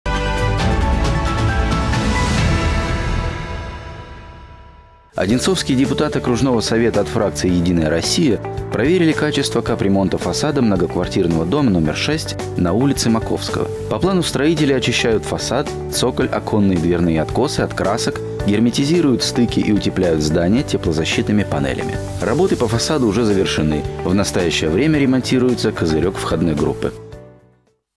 Капремонт на Маковского, 6
Одинцовские депутаты окружного Совета от фракции «Единая Россия» проверили качество капремонта фасада многоквартирного дома №6 на улице Маковского. По плану строители очищают фасад, цоколь, оконные и дверные откосы от красок, герметизируют стыки и утепляют здание теплозащитными панелями. Работы по фасаду уже завершены. В настоящее время ремонтируется козырек входной группы.